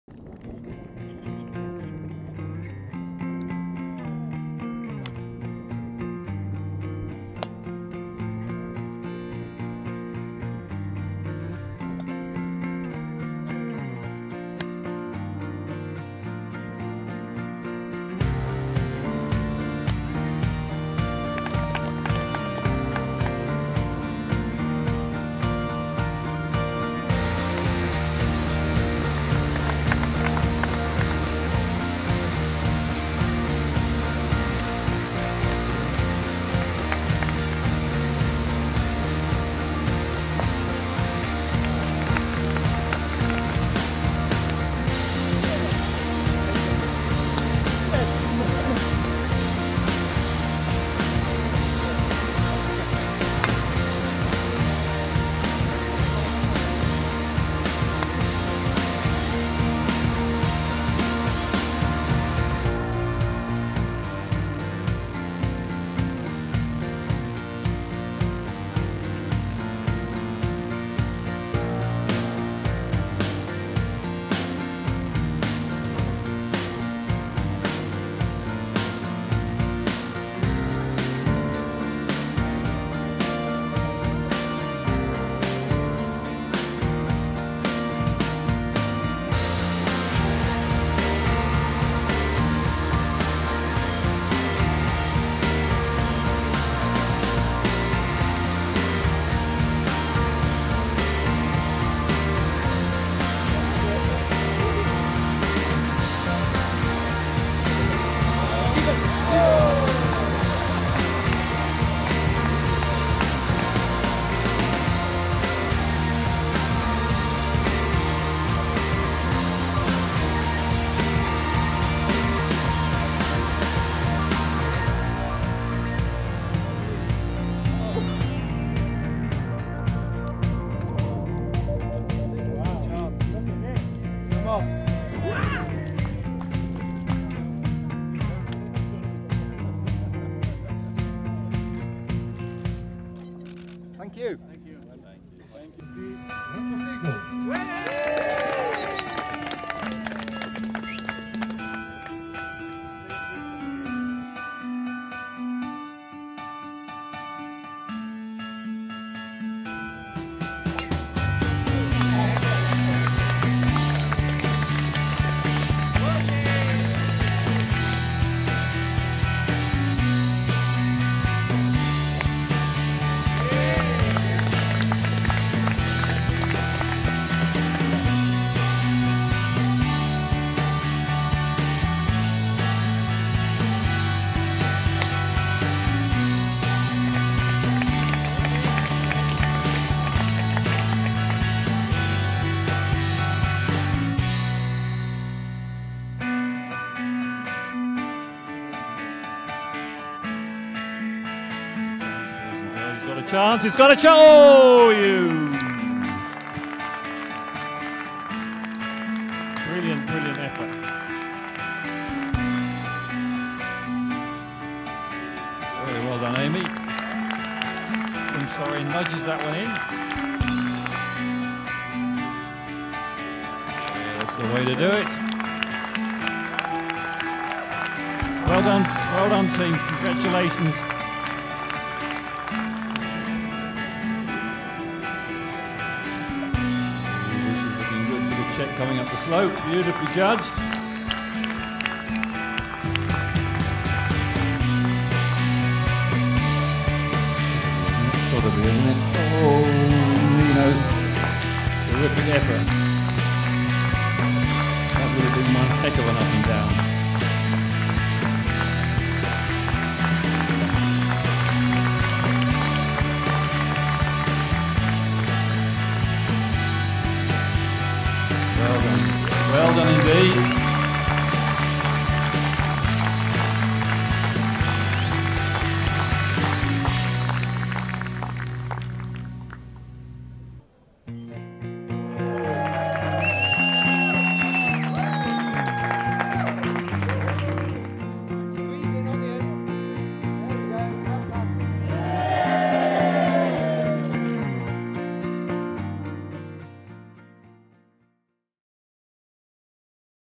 EDGA FULL copy
The European Tour's Portugal Masters 2017 had six EDGA players give a demonstration and play the 18th hole following the last group of the third round.